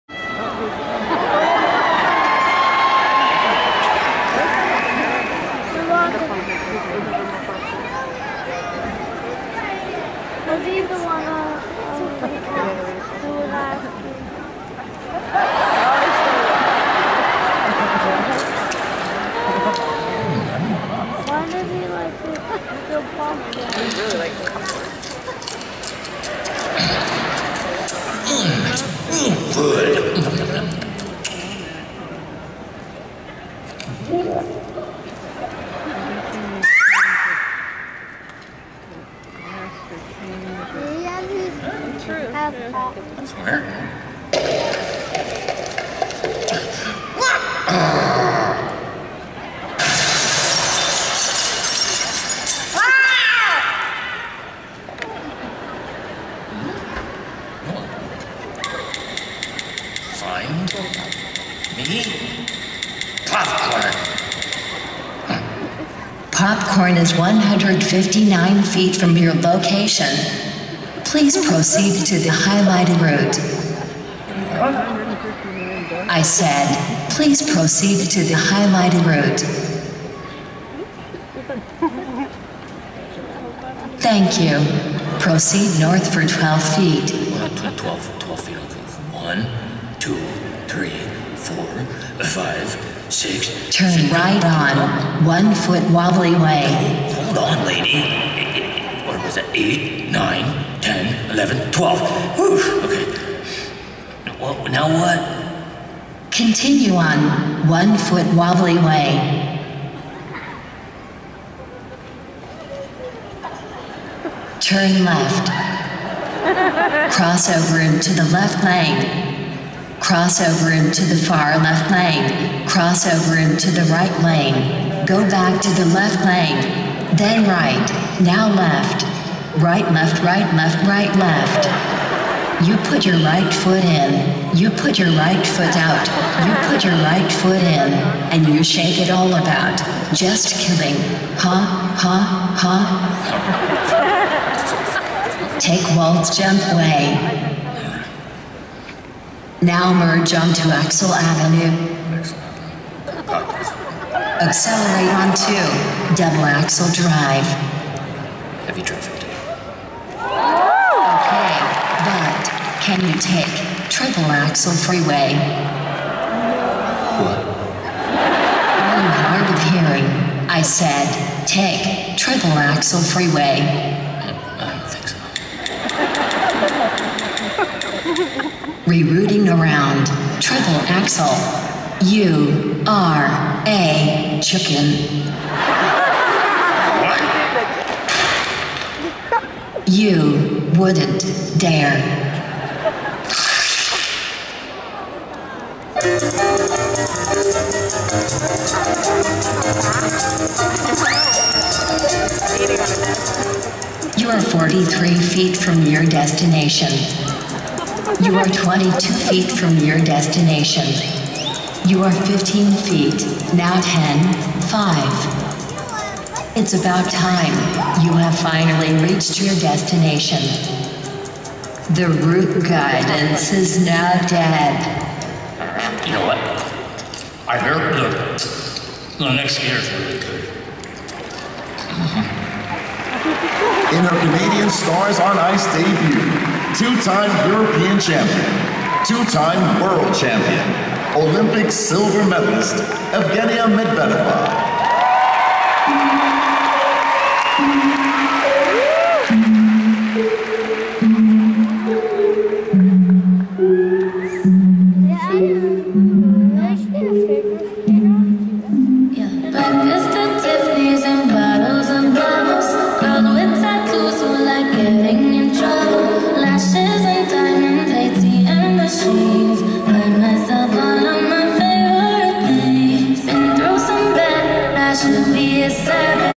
Popcorn Perils - Kurt Browning - Vancouver Stars on Ice 2019
Kurt Browning performs "Popcorn Perils" in the Vancouver Stars on Ice show on May 16, 2019 (fan video).